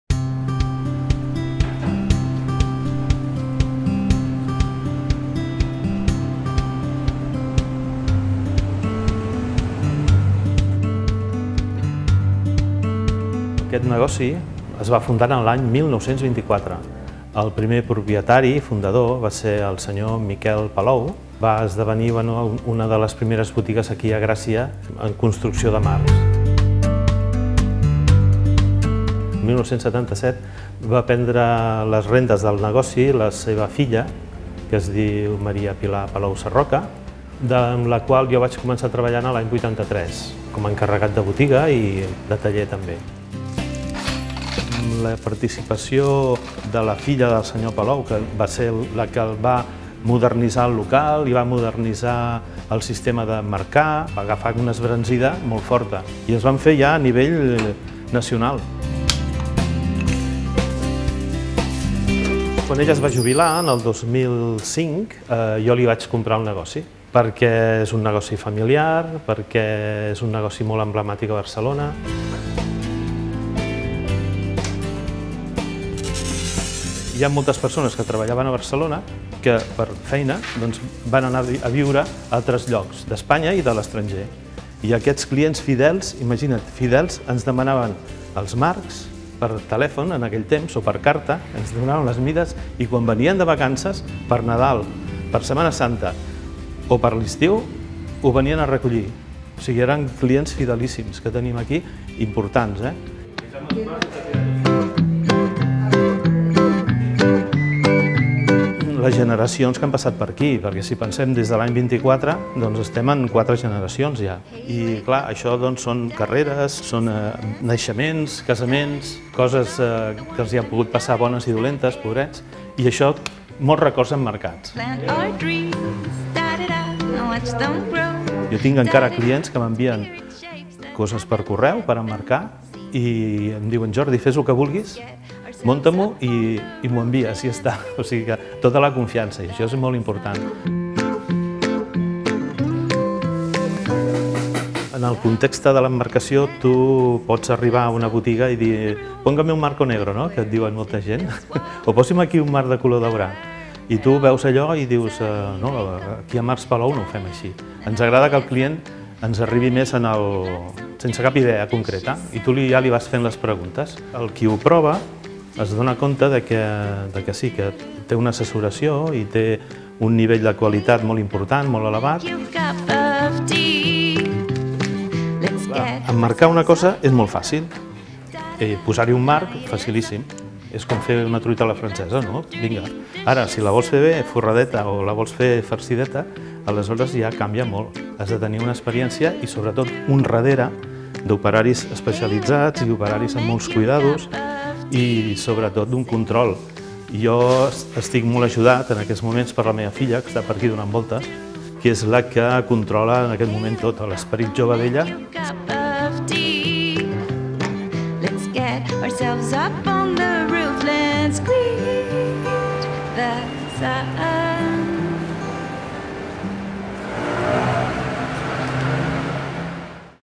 DARRERE EL TAULELL: "Marcs Palou"
Establiment referent per l'emmarcació de quadres, gravats, làmines i obra gràfica a mida, i que va obrir les seves portes al 1924. Més a